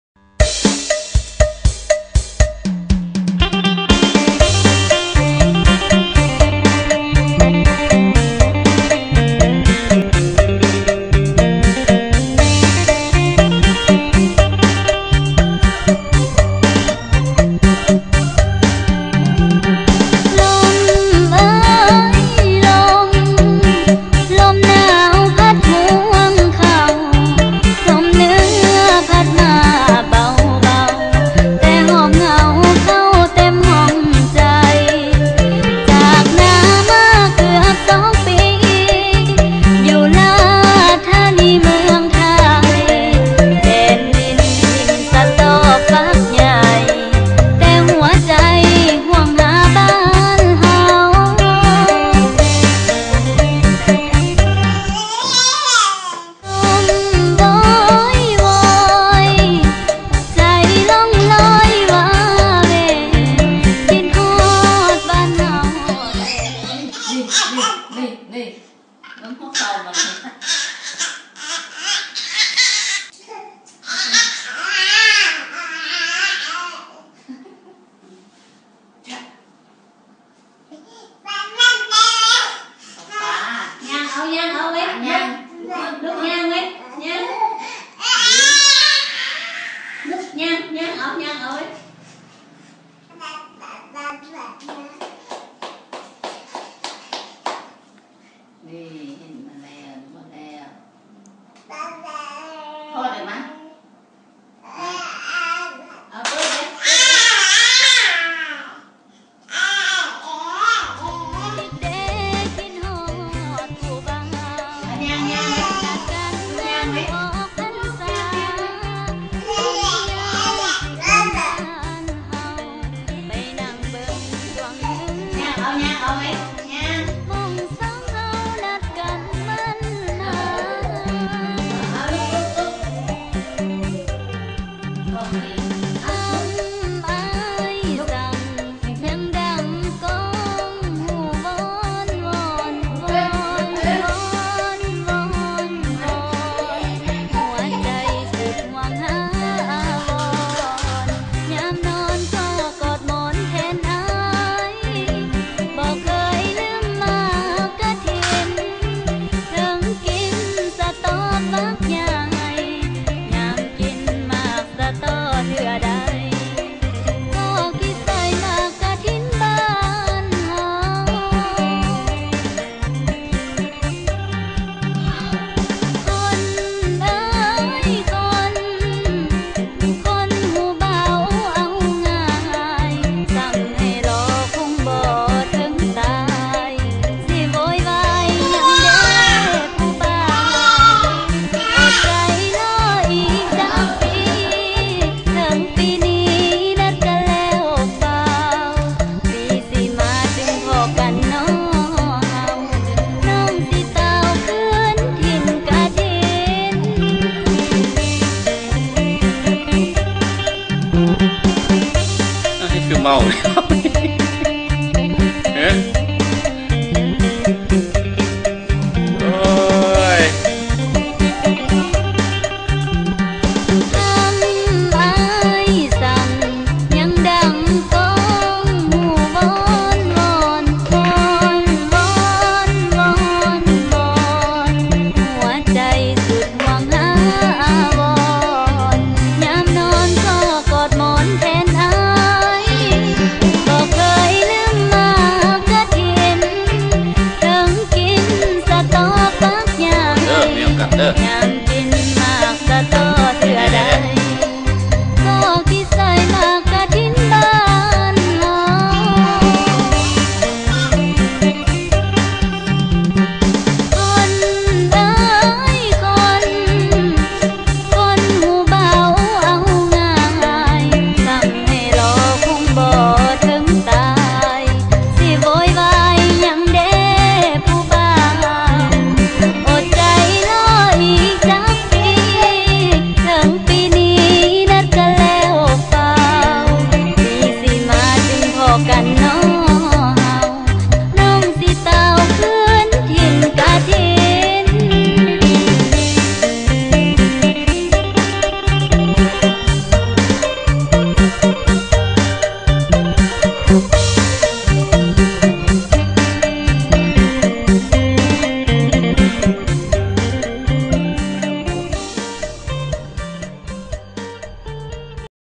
MN 4 2 2014
ມື້ນີ້ດີໃຈ ,, ຫຼານຍ່າງໄດ້ຫຼາຍກ້າວແລ້ວ